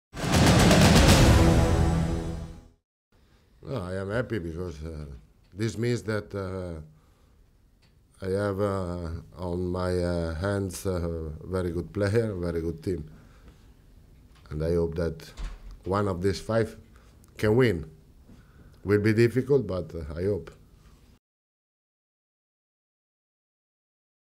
Chelsea reaction to Fifa's shortlist for World Player
Carlo Ancelotti gives his reaction to having five of his players on the Fifa shortlist for World Player of the Year.